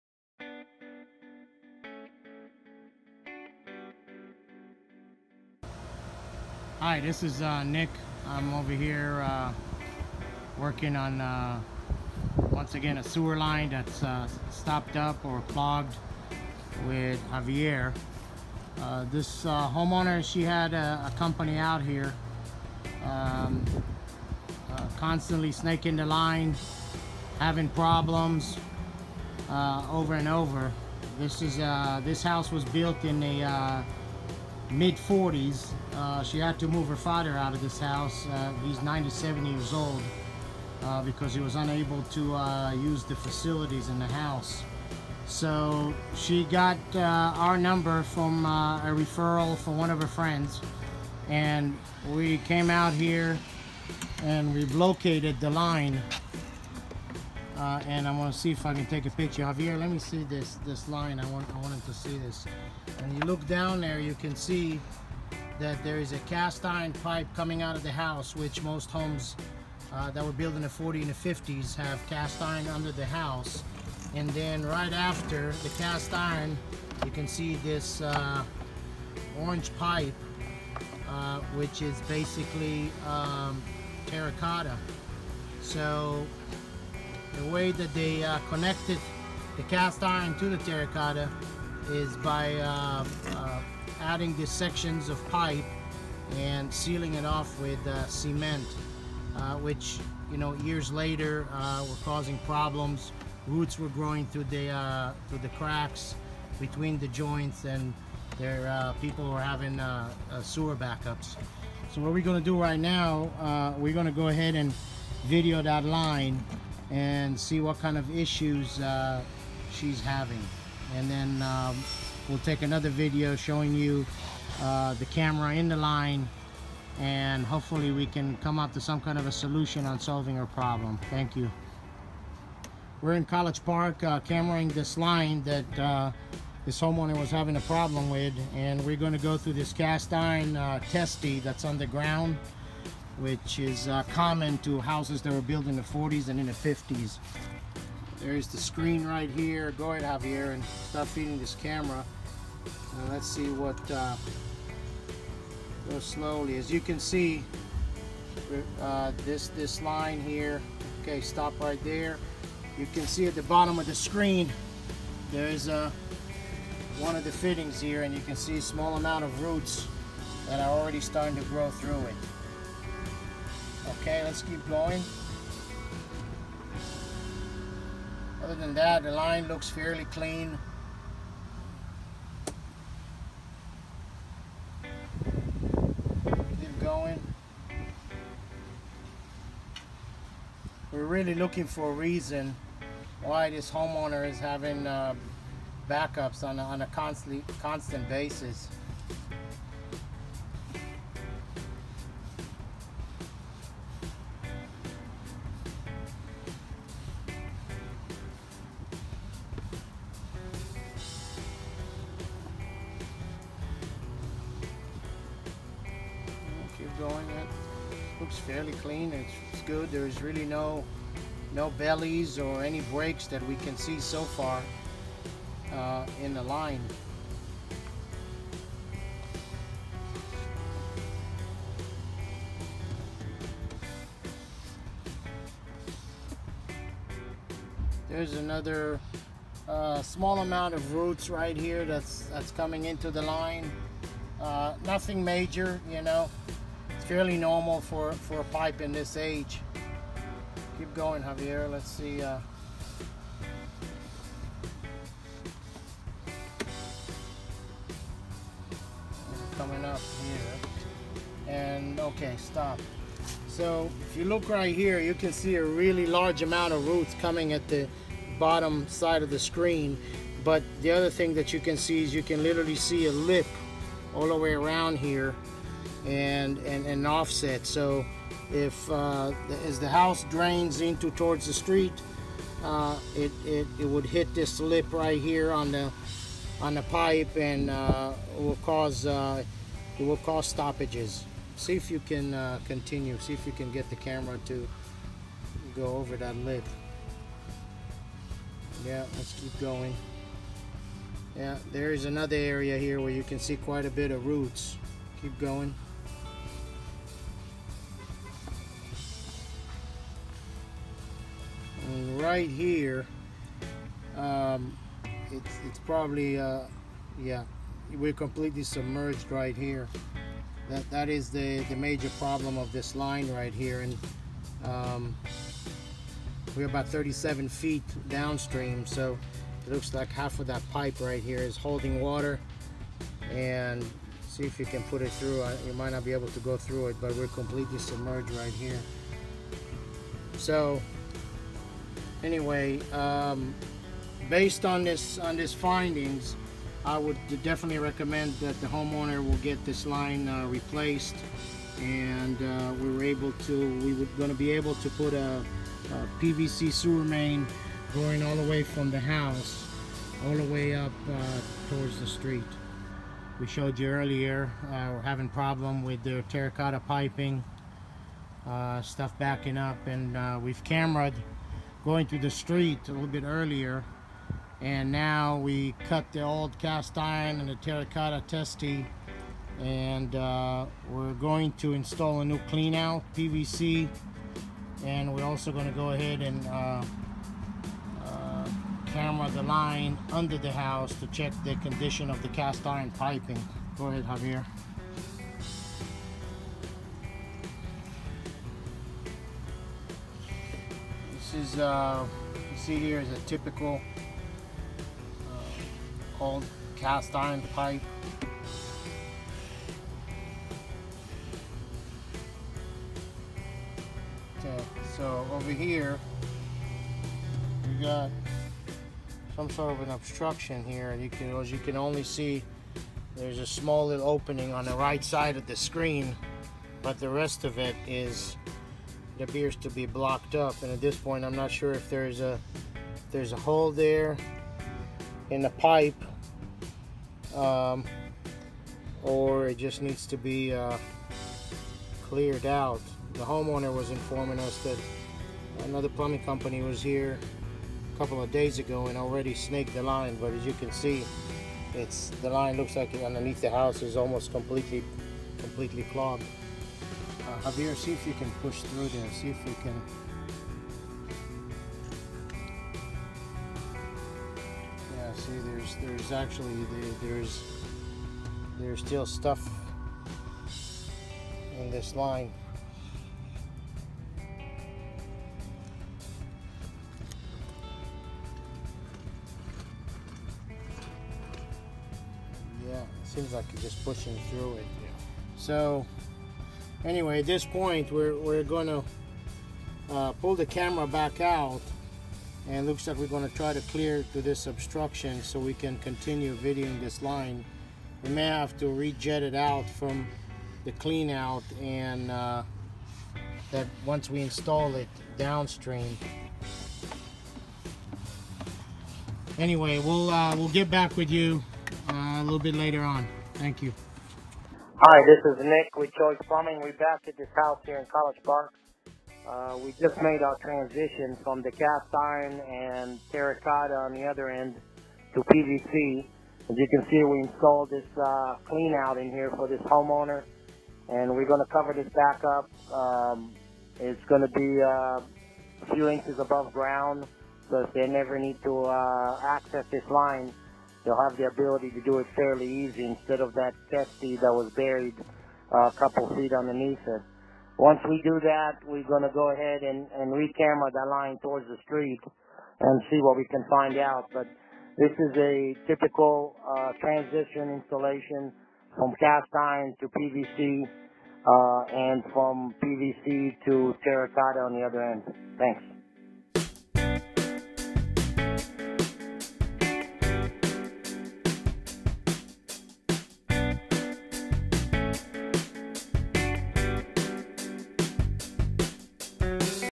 Clogged Sewer Line Repair | College Park Orlando
Choice Plumbing was called to an older home in College Park for a clogged sewer line issue that was giving the homeowner many headaches.  The homeowner had used a different plumbing company in the past that had snaked the line over and over without any permanent results.  So finally, after a referral from a good friend, the homeowner called Choice Plumbing.  We were able to use our state-of-the-art sewer cameras to take a look inside the pipe and figure out the "root" of the issue once and for all.  Once we discovered the real problem, we were able to replace the old and cracked terracotta pipes with strong PVC pipes that will not be penetrated by tree roots in the future.  Check out the video of the job, and see actual footage from inside the pipe where you can see the roots invading the pipe and causing the backups.